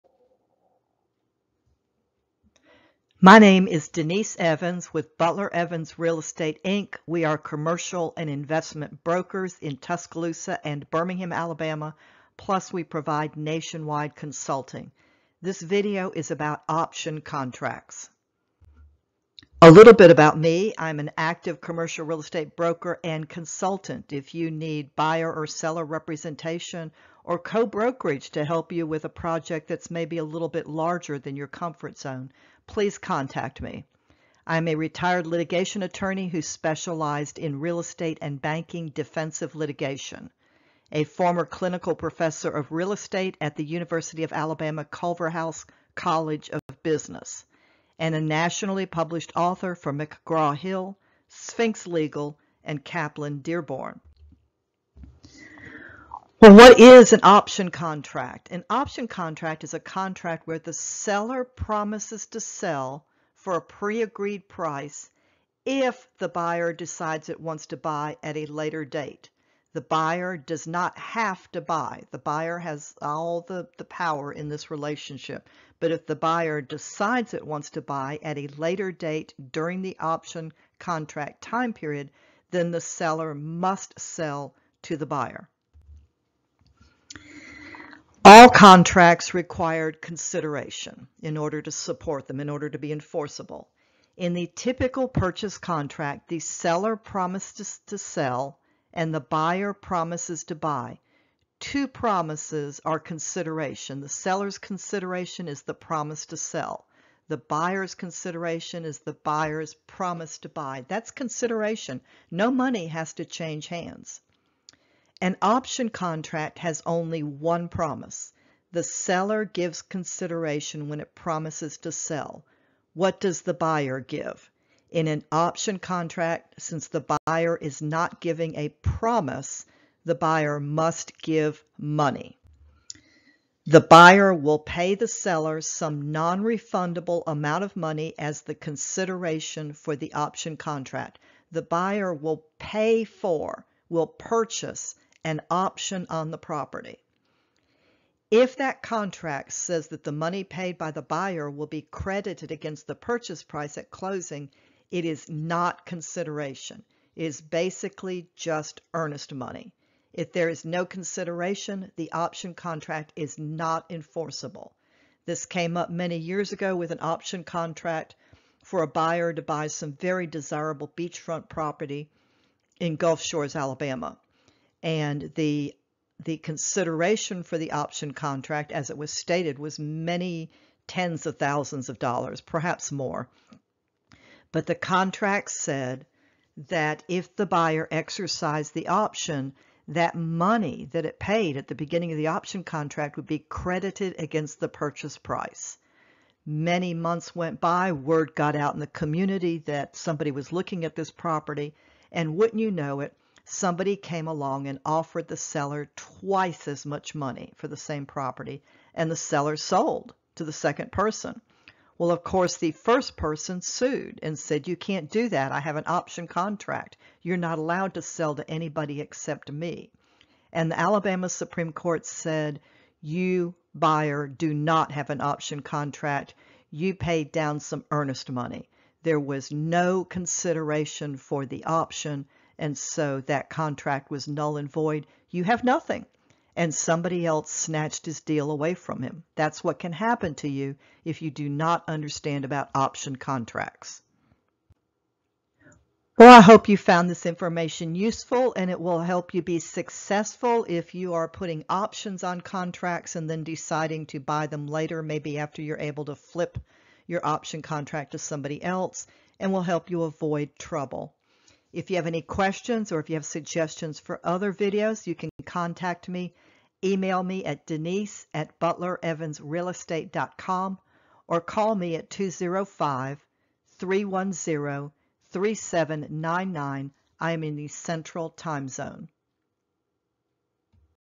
Option Contracts
If you use option contracts to buy or flip real estate, this video explains a technicality that could result in someone else getting your deal, if you don't know how to structure the option contract correctly.